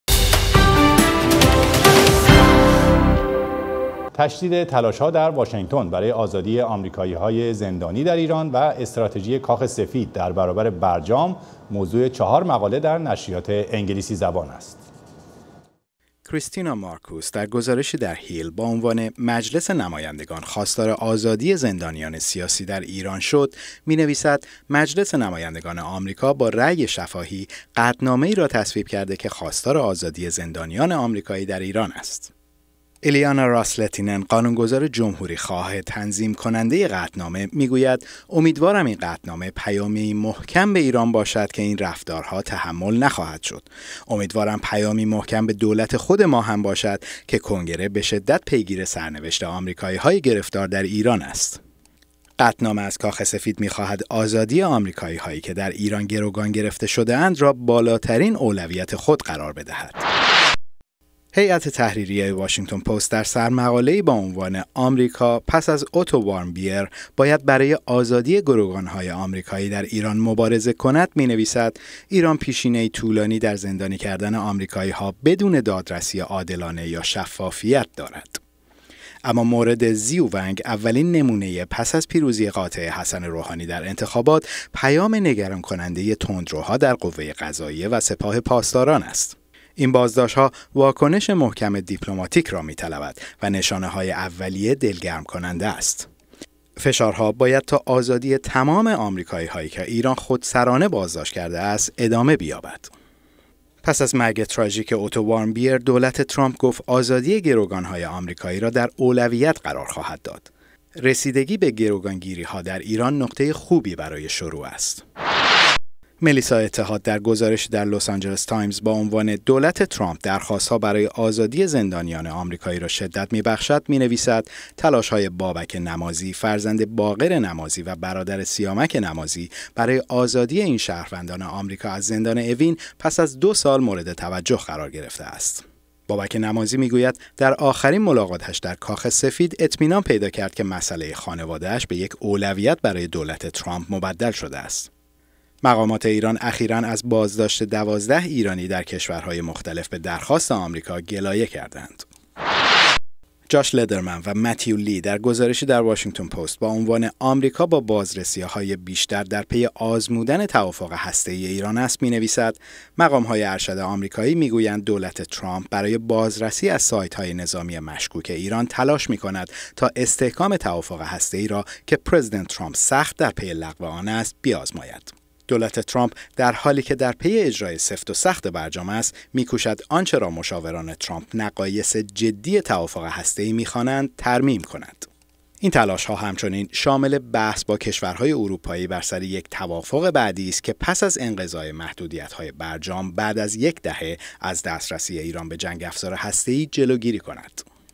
نگاهی به مطبوعات: استراتژی کاخ سفید در برابر برجام
تشدید تلاش ها در واشنگتن برای آزادی آمریکایی های زندانی در ایران و استراتژی کاخ سفید در برابر برجام موضوع چهار مقاله در نشریات انگلیسی زبان است.